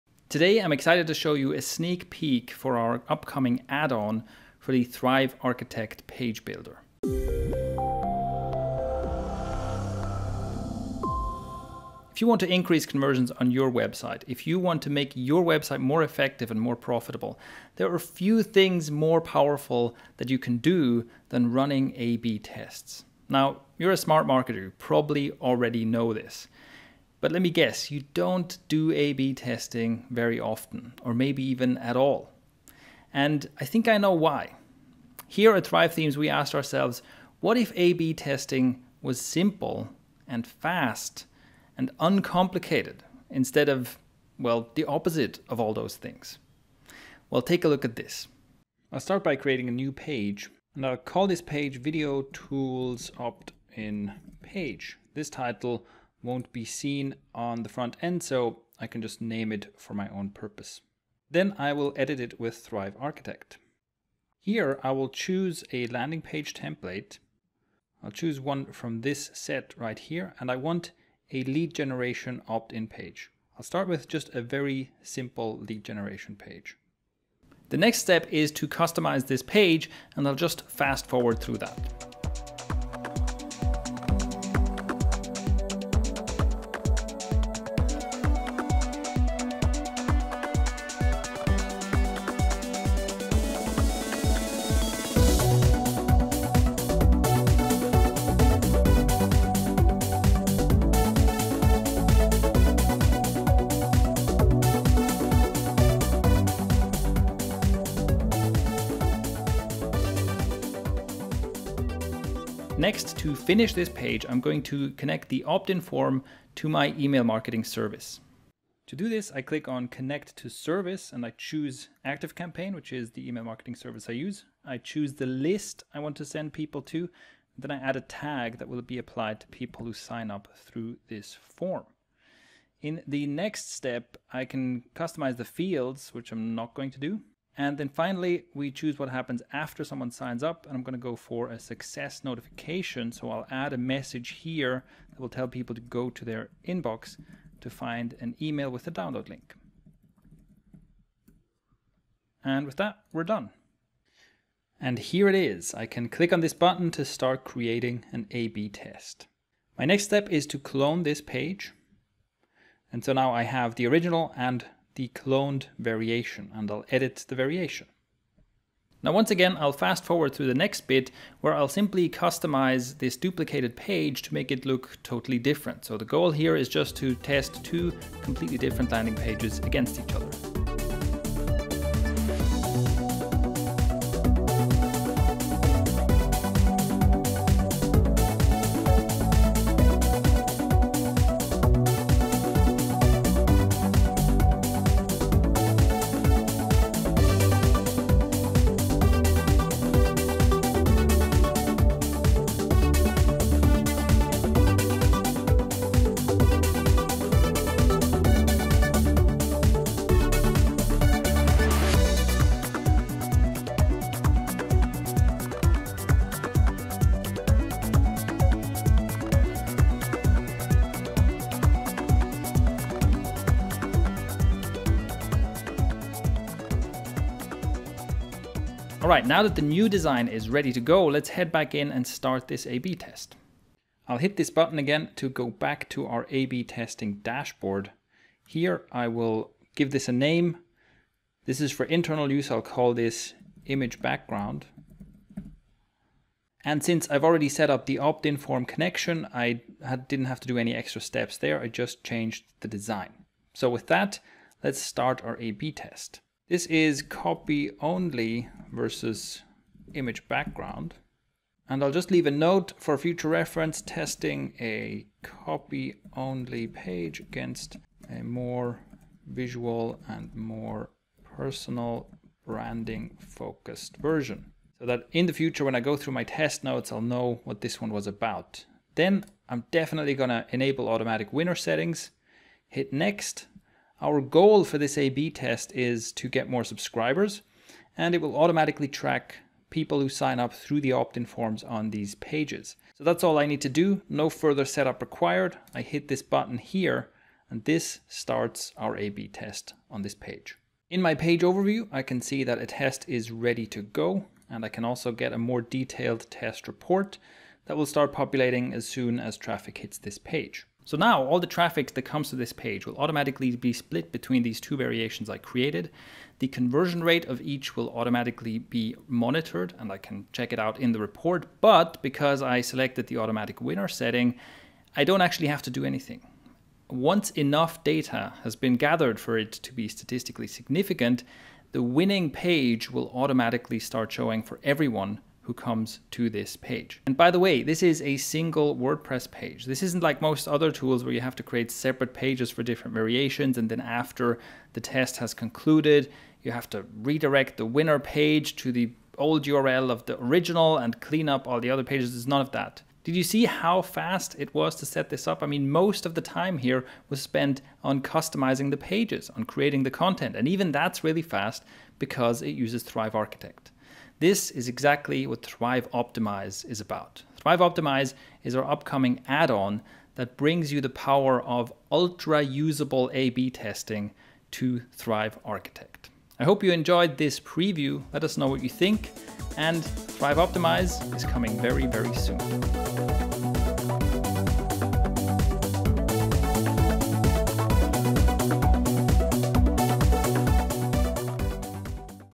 First Look at Our Next Conversion Boosting Product...
We're about to release an add-on for the Thrive Architect page builder. Here's the first look at what it does.